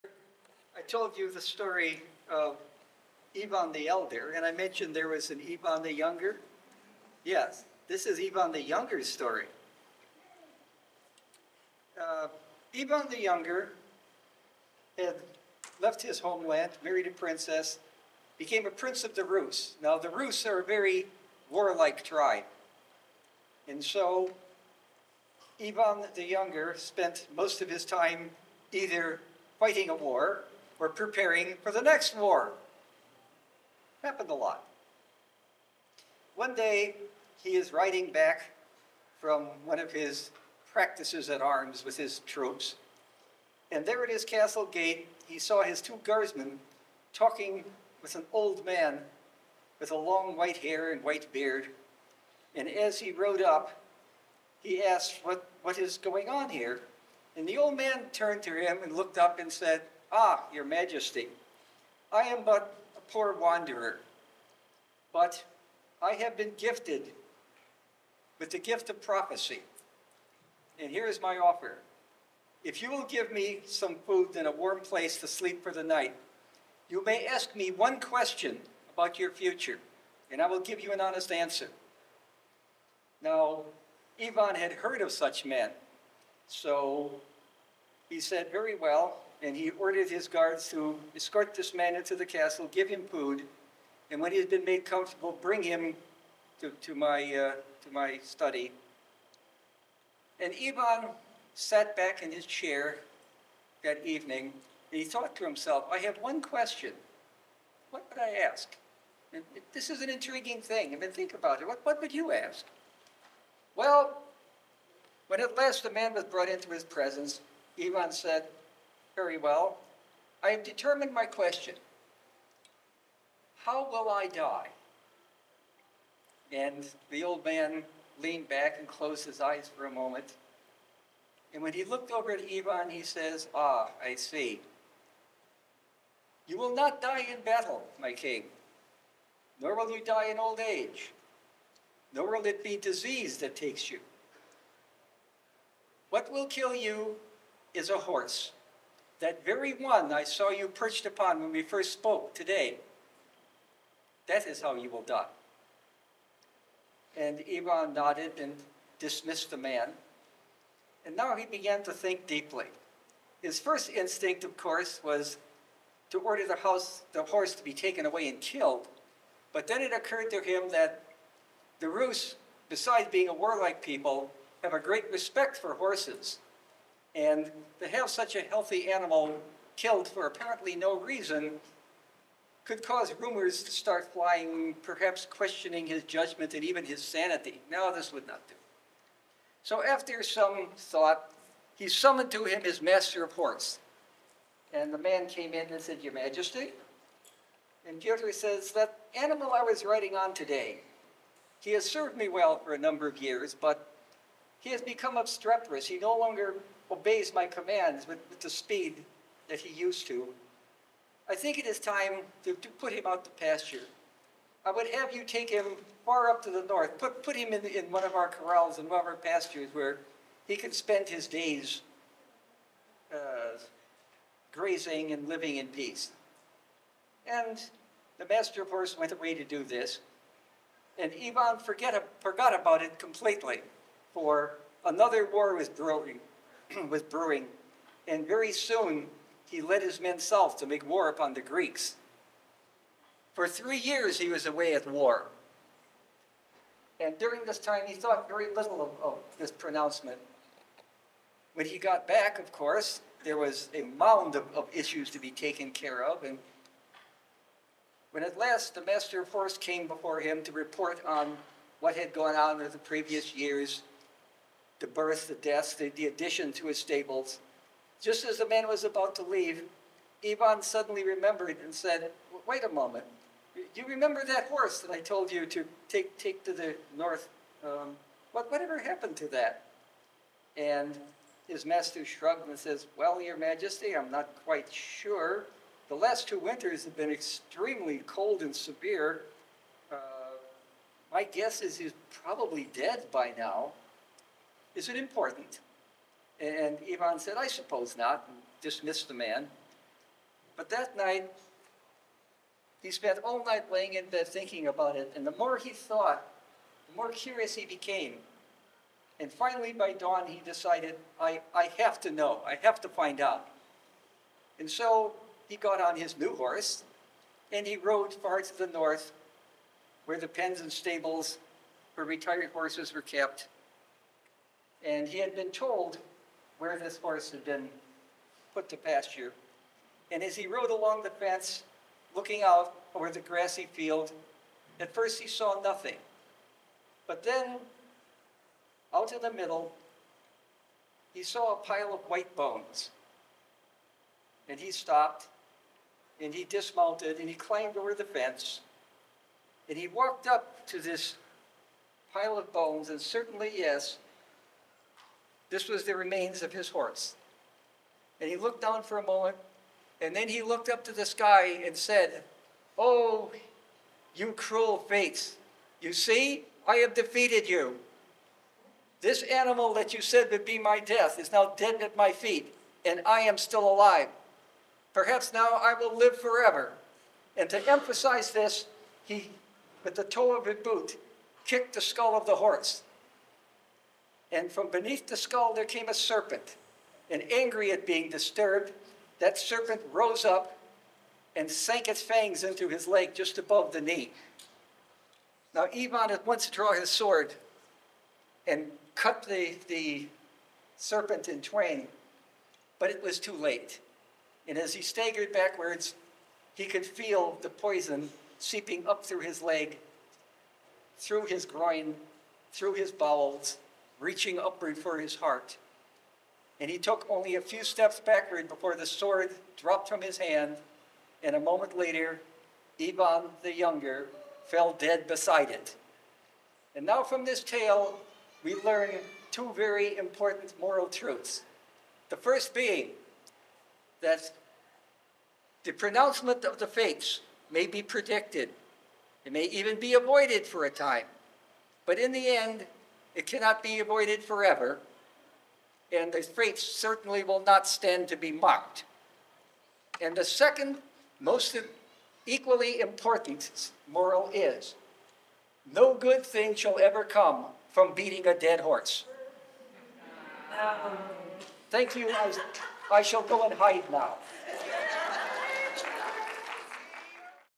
The Moral is Oral: Ivan the Younger - Ellis von Oesterreich
The Moral is Oral:
Celebrate the ancient story, told by firelight as the stars come out. Extra points for bringing it straight from your memory - no book (of any sort).